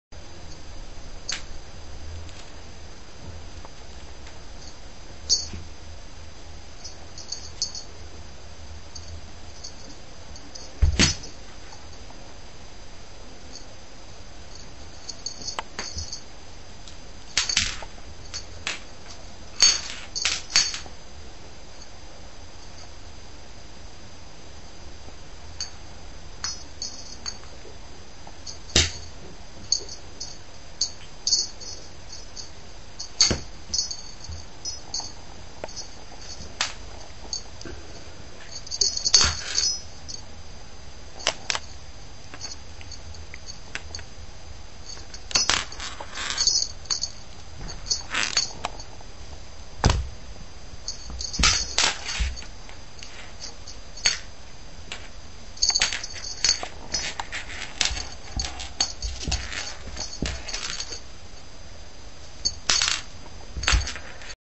Dola and the feather
Dola has traded in her mouse toy for the more poche feather and bell combination.   She truly enjoys the bouncy parts of life!